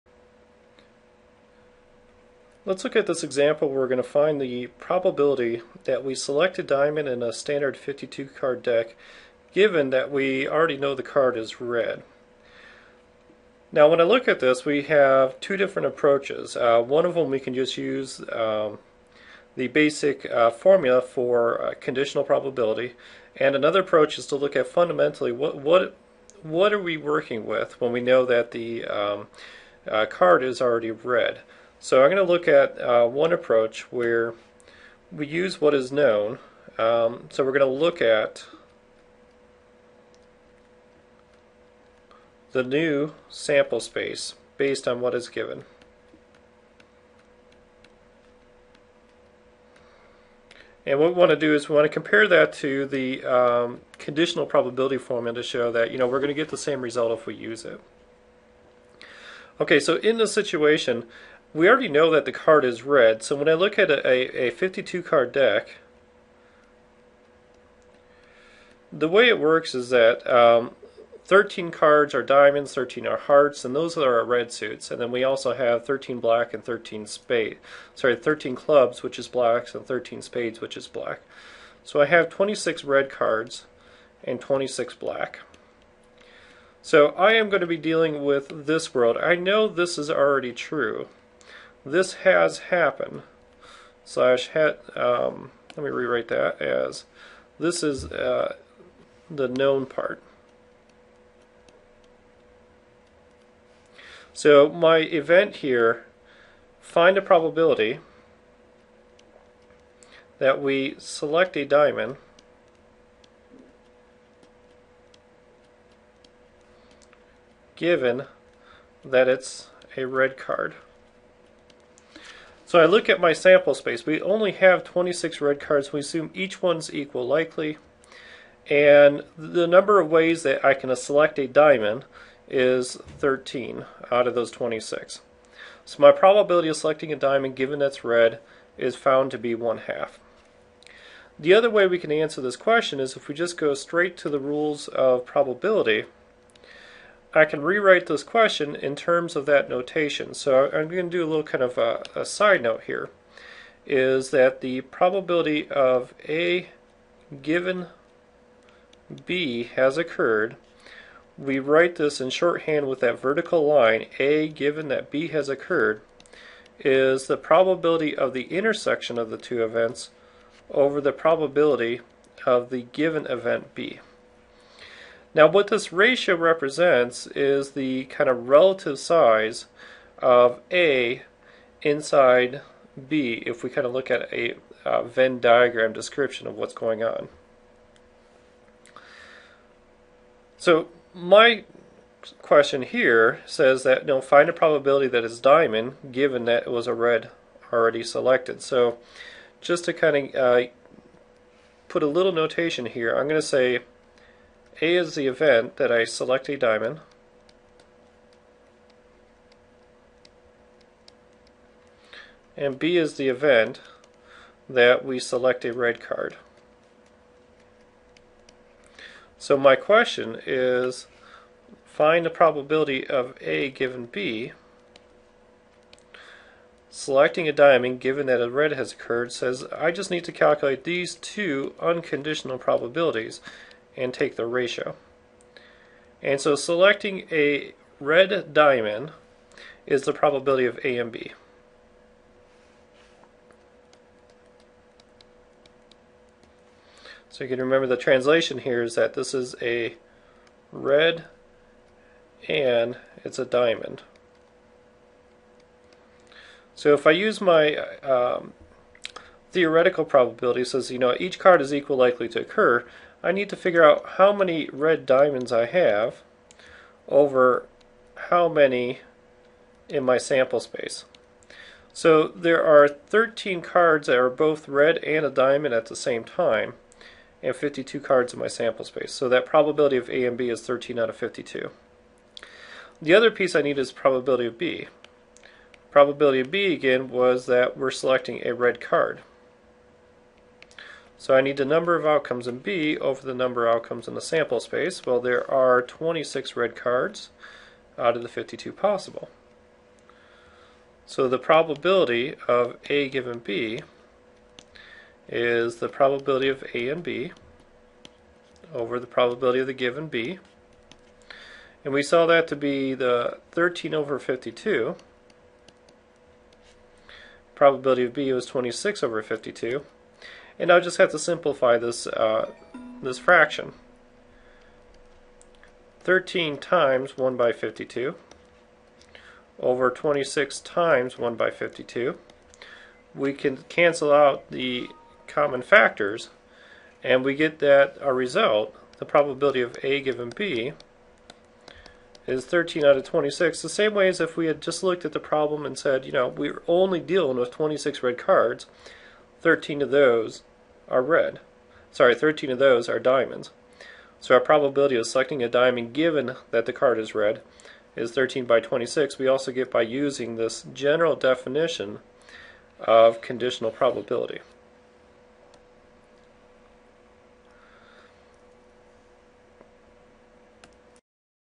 Probability of selecting a diamond given a red card
Find the probabililty that we select a diamond in a standard 52 card deck given we already know the card is red.